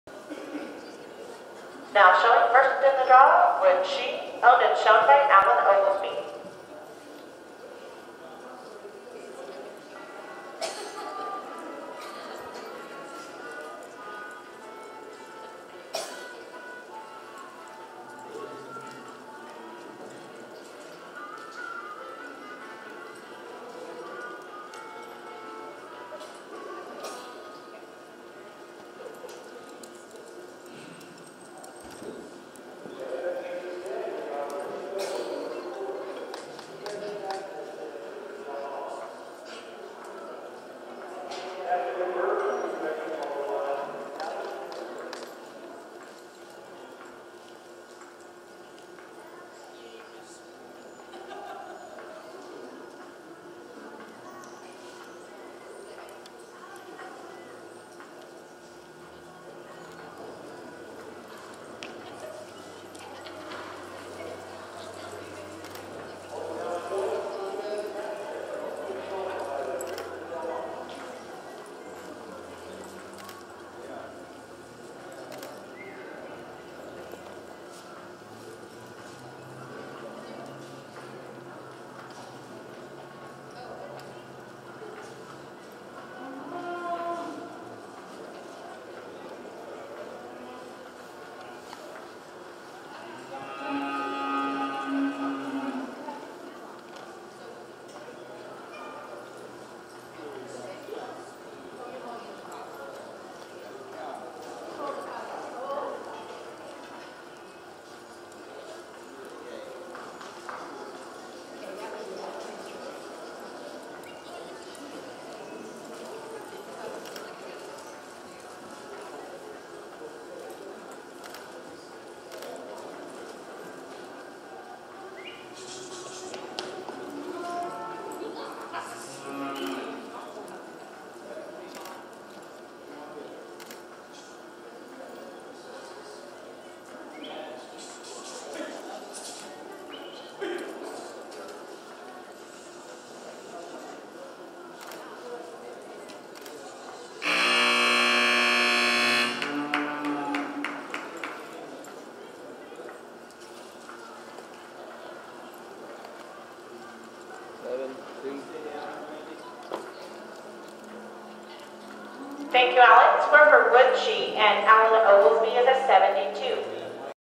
Wood She
Wood She owned and shown by Alan Ogelsby  RECHA January 3, 2020  NP Derby  Score 72